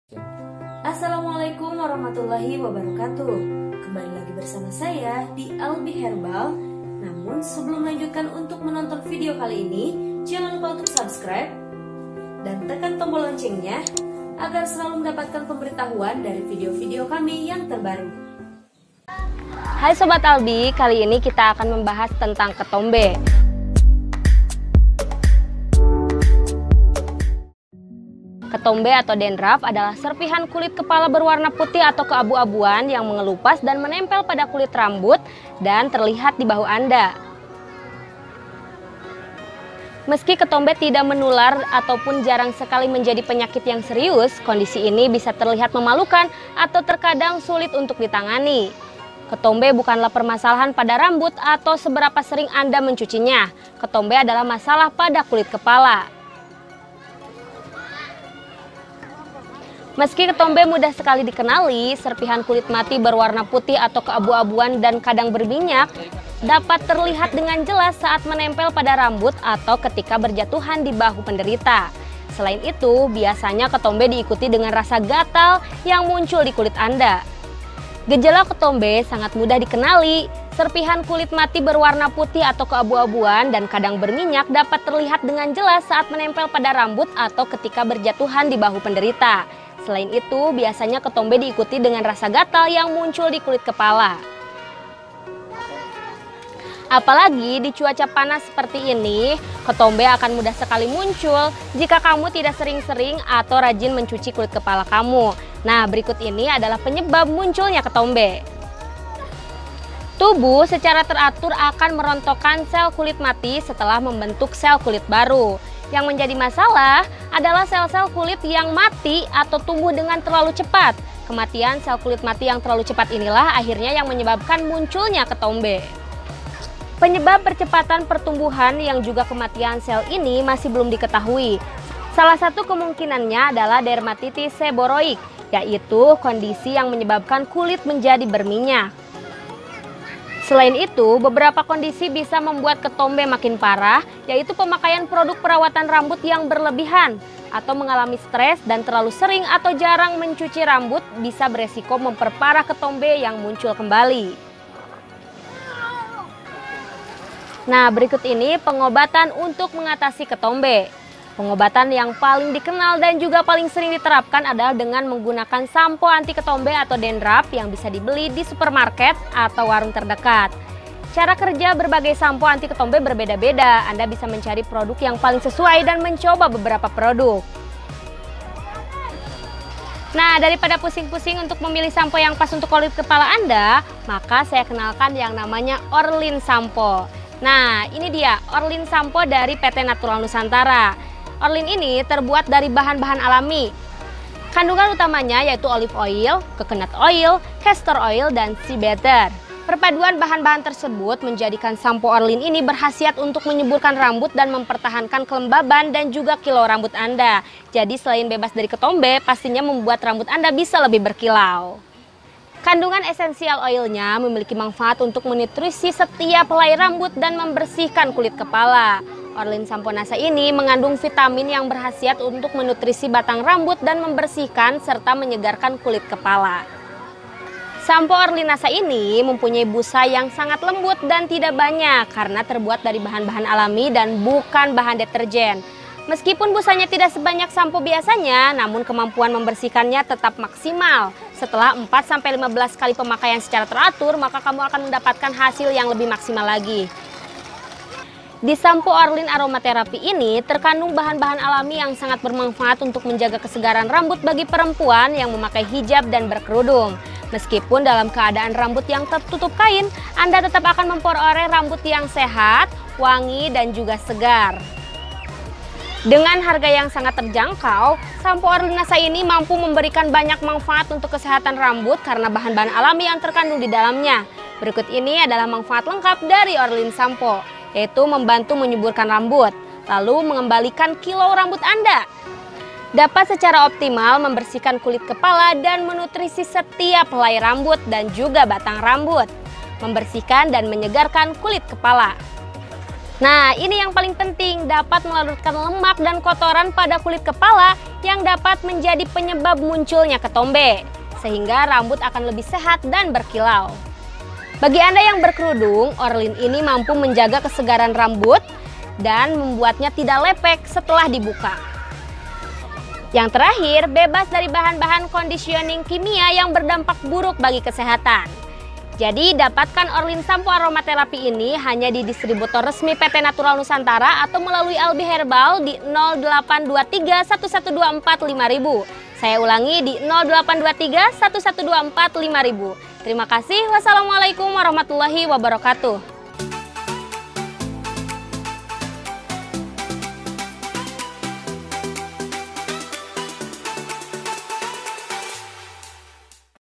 ORLYN Shampo Aromatherapy Asli NASA Untuk Menghilangkan Ketombe Rambut Yang Membandel
ORLYN Shampo Aromatherapy Asli NASA Untuk Menghilangkan Ketombe Rambut Yang Membandel Secara Alami.

ORLYN Shampo NASA ini dibuat dari bahan-bahan alami. Kandungan utamanya yaitu  olive oil, coconut oil, castor oil dan shea butter. Perpaduan bahan-bahan tersebut menjadikan Sampo ORLYN berkhasiat untuk menyuburkan rambut dan mempertahankan kelembaban dan kilau alami rambut.

Kandungan essensial oilnya memiliki manfaat untuk menutrisi setiap helai rambut dan membersihkan kulit kepala. ORLYN Shampoo NASA juga mengadnung vitamin yang berkhasiat untuk menutrisi batang rambut dan membersihkan serta menyegarkan kulit kepala.

Shampo ORLYN ini merupakan satu dari serangkaian perawatan rambut ORLYN Aromatherapy dari PT Natural Nusantara (NASA).  Satu paket perawatan rambut ORLYN Aromatherapy yang terdiri dari Shampo, Conditioner dan Hair Tonic merupakan produk perawatan rambut yang terbaru dari PT NASA yang baru dirilis pada Desember 2017 ini.